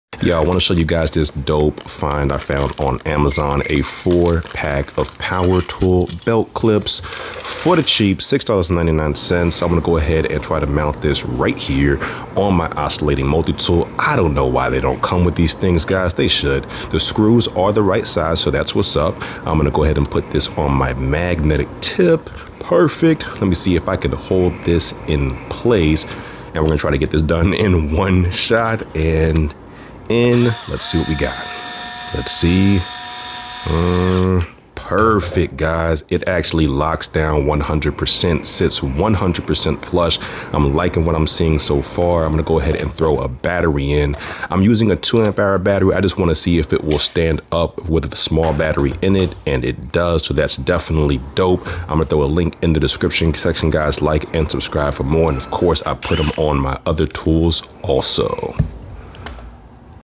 TOOL BELT CLIPS THAT ACTUALLY WORK!
In this short, I show you a great find from Amazon. We all know that most tools do not come with BELT CLIPS for whatever reason. Well, problem solved! I found this 4pack for super cheap and these are actually some nice quality belt clips/Hooks!
Check the community post for the link!